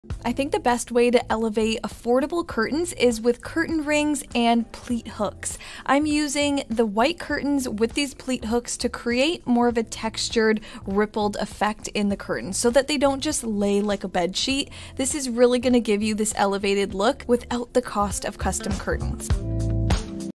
How To Make IKEA curtains LOOK EXPENSIVE!
Transform your space in an instant with this easy and cost-effective hack! Curtain rings and pleat hooks can give any affordable curtains a luxurious, textured look. See how I used them to add more depth to my white curtains without breaking the bank! These are the IKEA Rivta curtains!